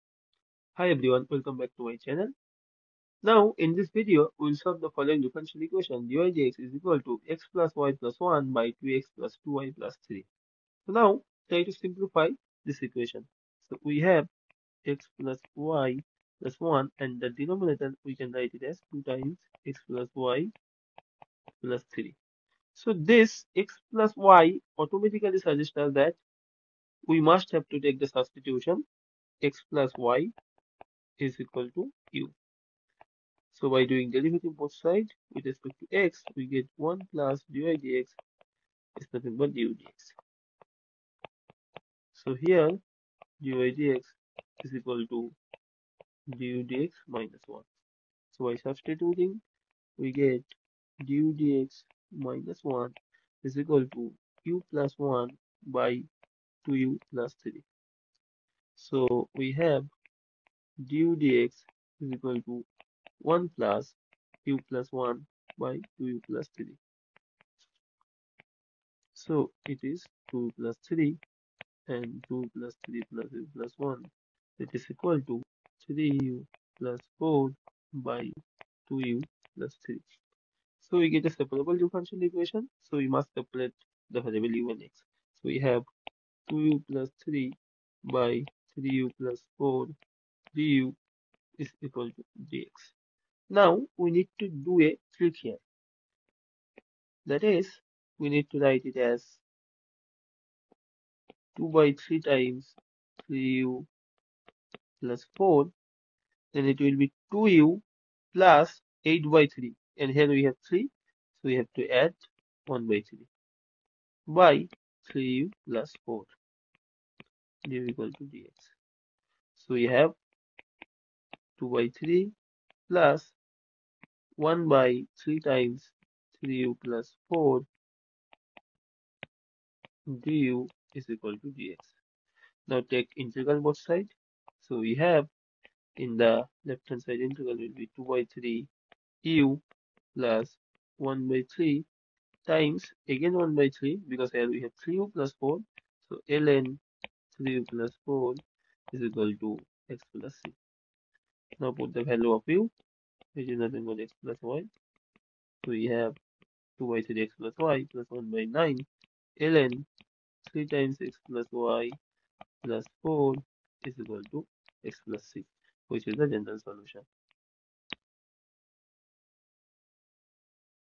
Solve: dy/dx= (x+y+1)/(2x+2y+3)|| 1st order ODE|| Seperable ODE
In this video, we will solve the ODE:
dx/dy= (x+y+1)/(2x+2y+3). Here we made a suitable substitution x+y=u and made this equation into a first order seperable differential equation.